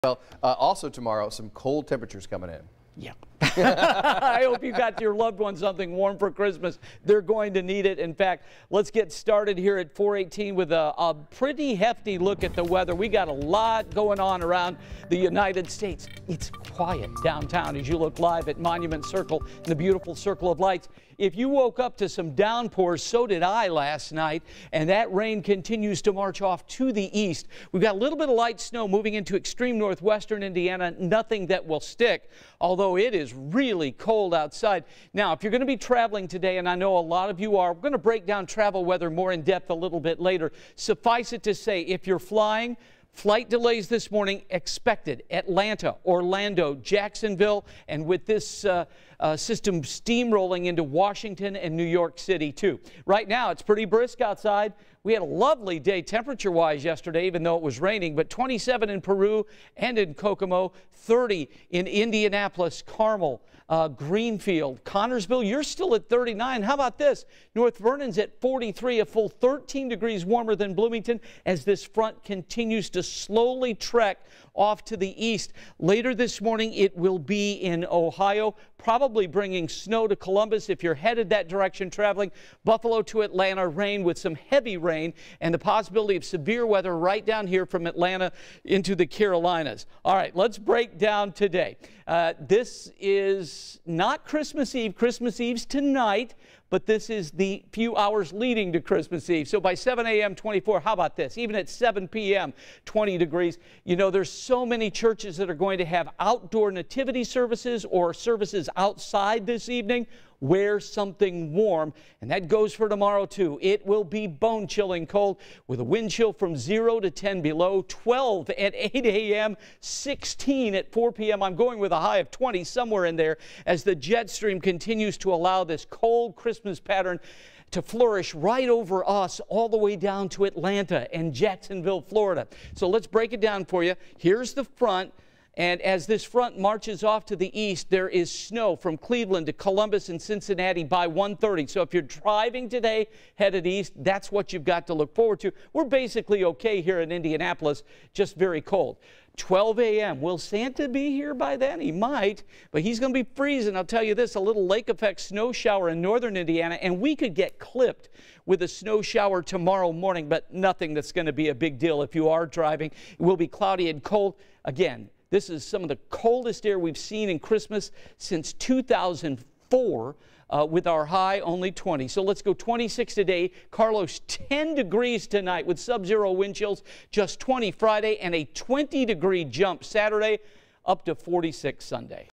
Live Doppler 13 Weather Forecast — 12/24/2020 Sunrise update
Look for a couple of flurries dancing around the area today with a high in the upper 20s and a bitter cold 10 degrees tonight.